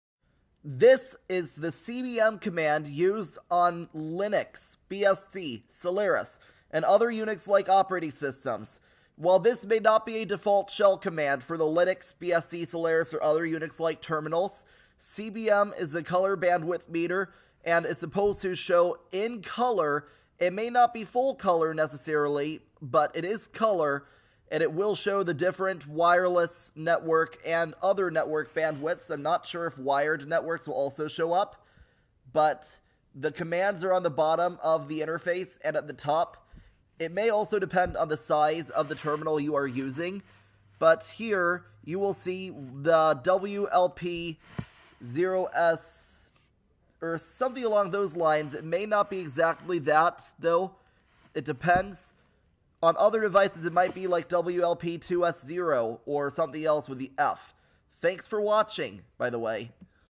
Color Bandwidth Meter (cbm) Demo for Linux
Created with FFmpeg and Shotcut on Ubuntu 24.04.2 LTS. novaTopFlex demonstrates the `cbm` (Color Bandwidth Meter) command with functionality to showcase primarily hardware network speeds. Interfaces may be wired, wireless, virtual, or otherwise determined.